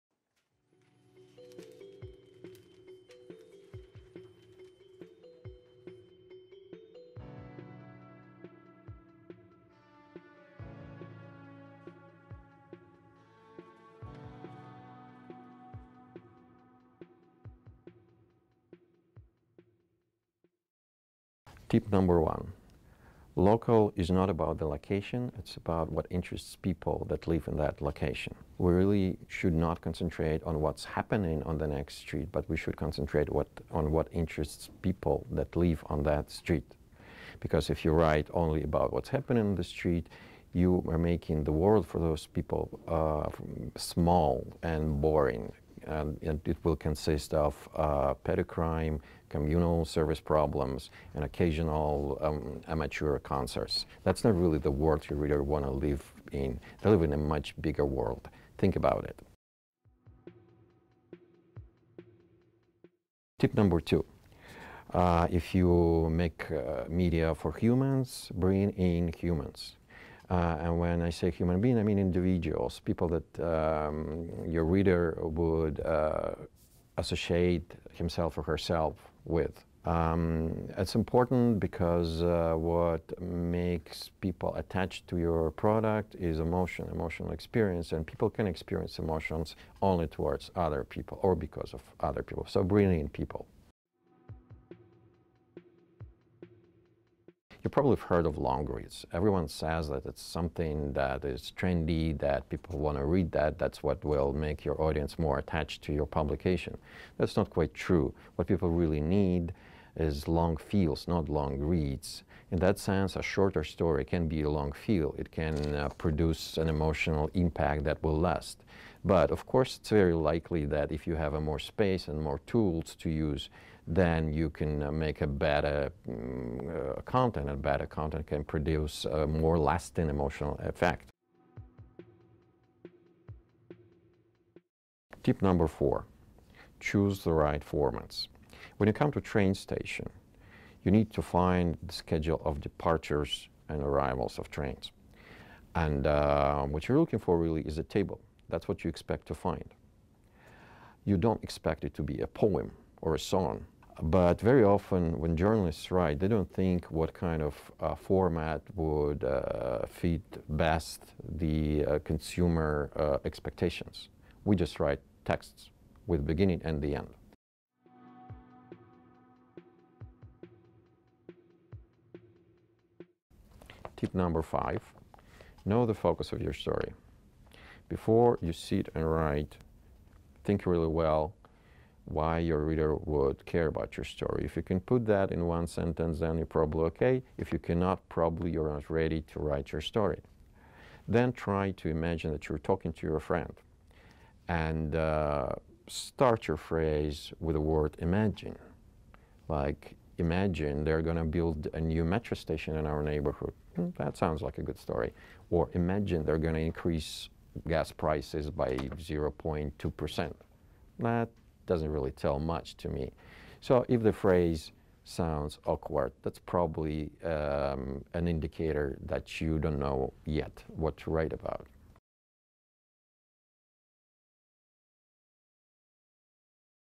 Še pieci: Dmitrijs Surnins
Pieci padomi no pieciem Baltijas Mediju izcilības centra treneriem.
Dmitrijs Surnins, neatkarīgais mediju konsultants.